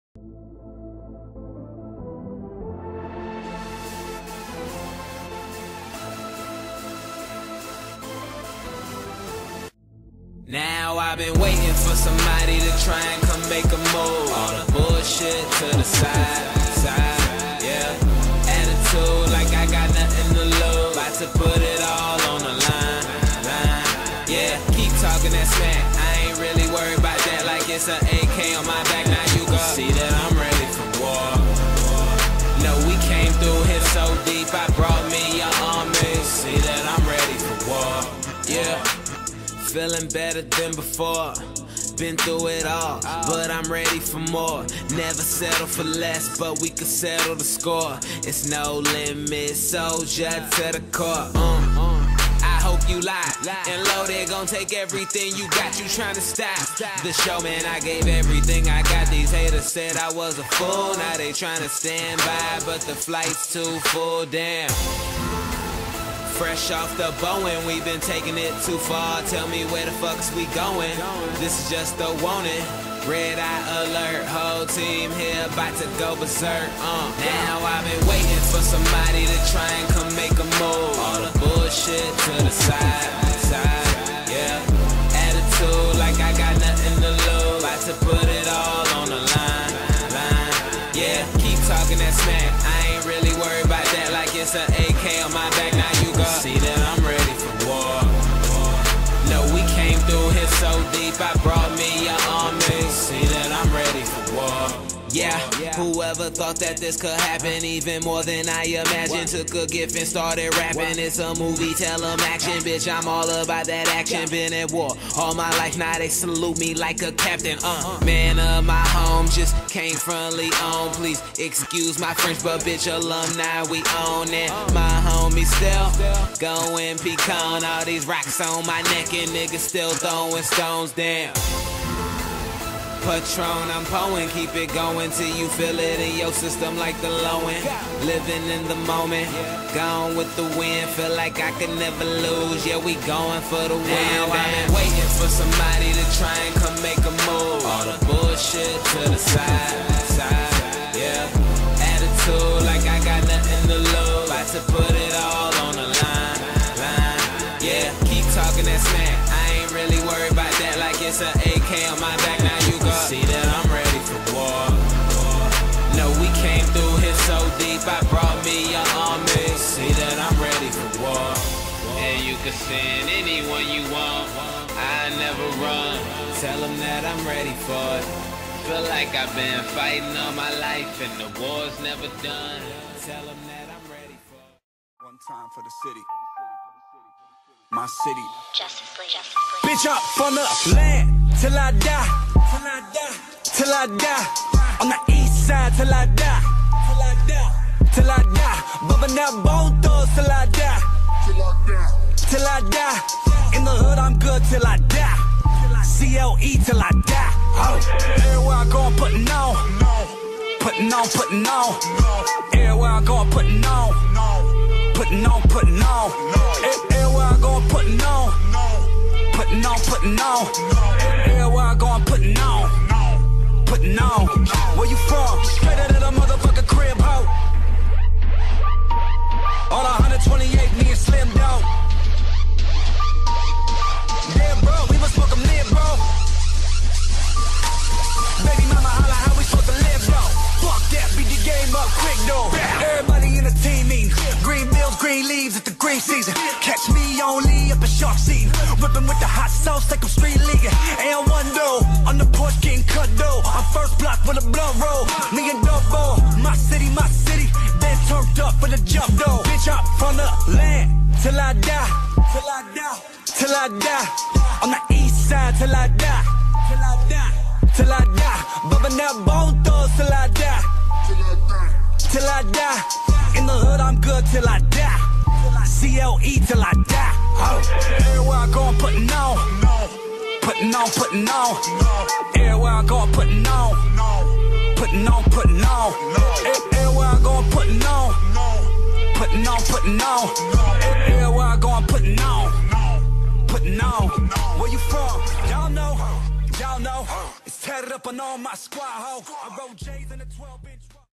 Jalen Mayden || "The Moose" || Highlight Mix
Top Dual Threat QB for 2018
Height-6'2
Weight-210
#4 | QB | Sachse High School
Sophomore Season
Story Book Films